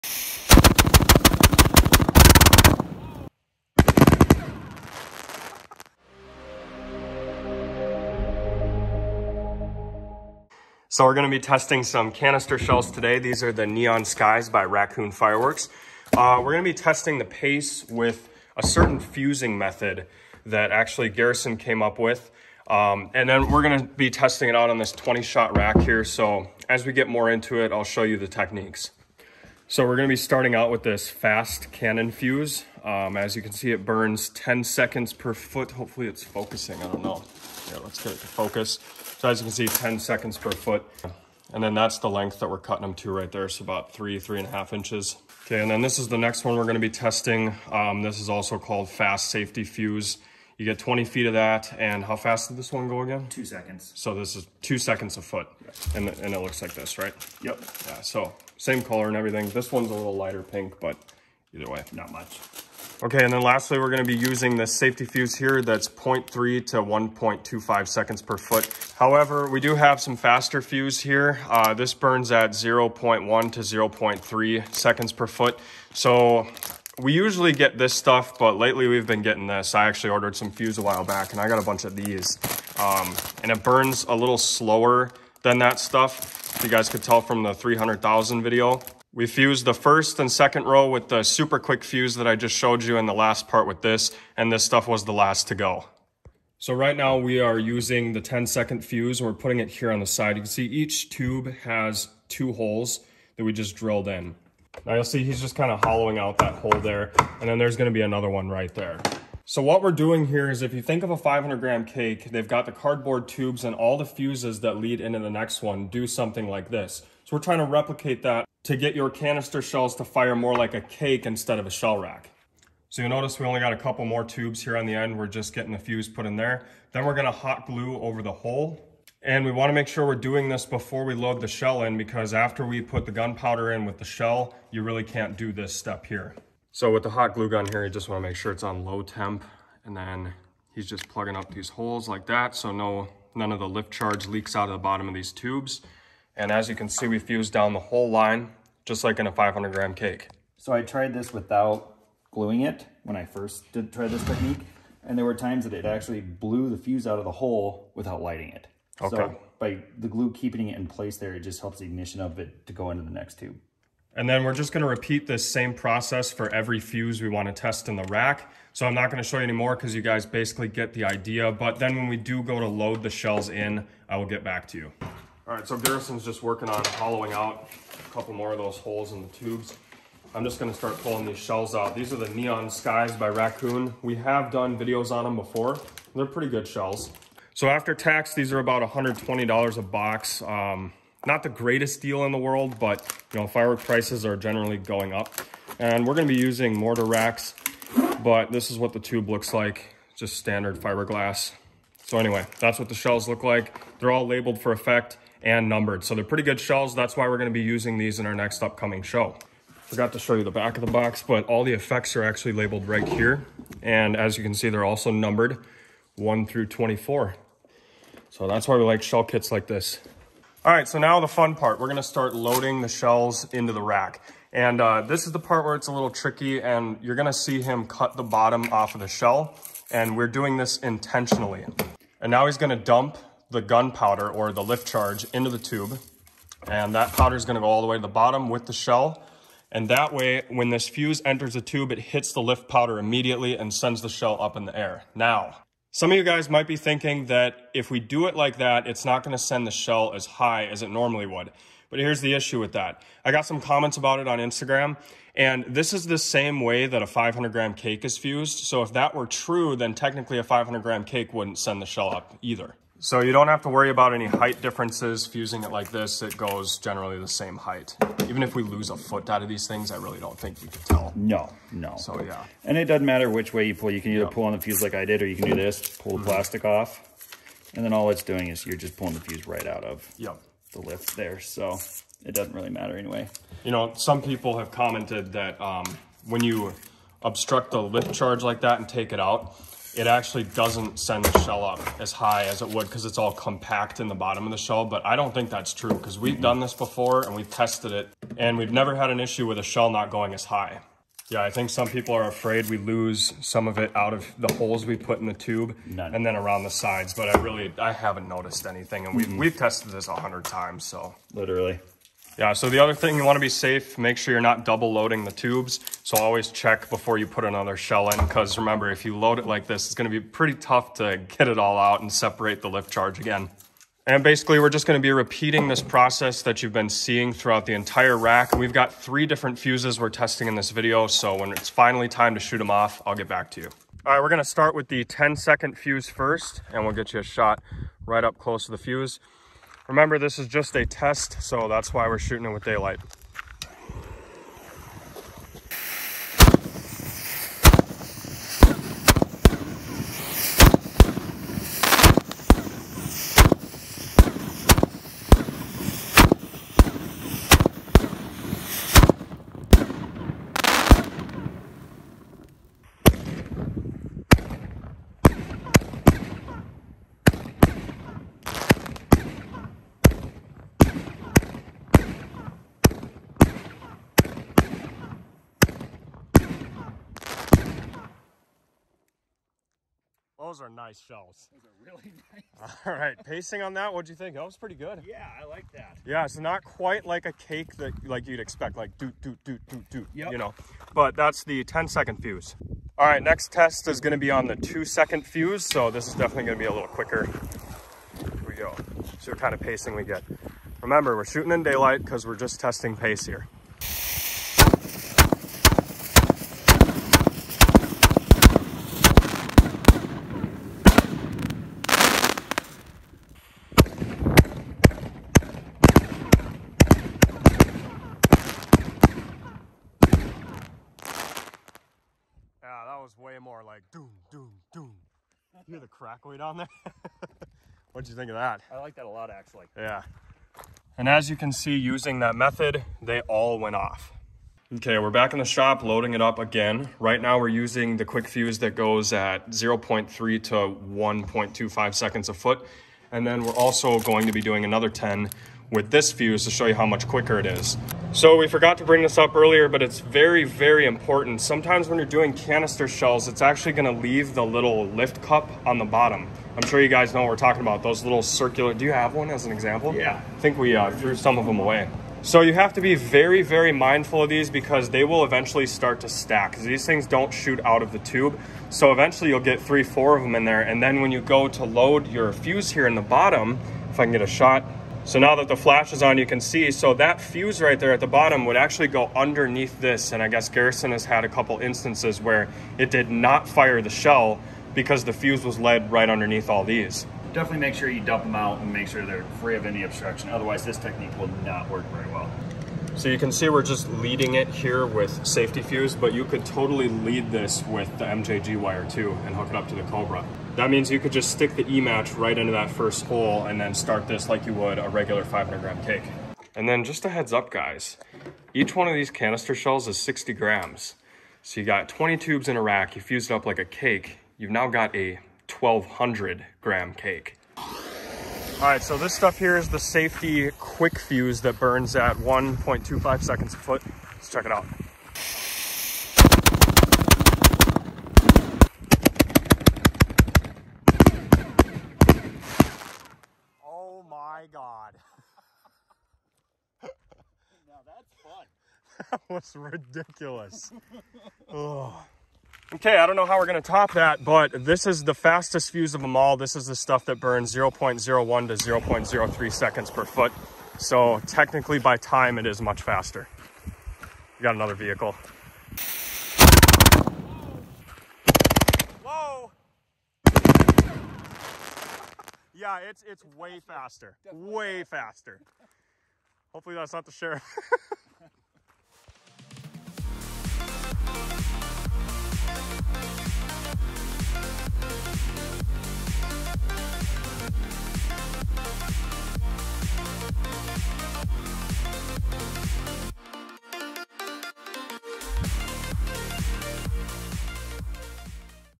Testing Canister Shells & Fusing Techniques to Fire like a Cake
After our tests we can confirm that the only inconsistency lies within the time-fuse built into the shell itself. That is out of our control. What IS in our control is the pace at which the shells leave the mortar rack tubes. We started with the 10 second a ft fuse then the 3 second a ft fuse. Then, we tested the 0.03–1.25 second a ft quick fuse and finally the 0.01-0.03 second super quick hobby fuse. I think our favorite pace was definitely the 1.25 second quick fuse since the shells exited the tube at the same pace as a 1/1 cake would fire. Hope this helps and hope you all enjoyed!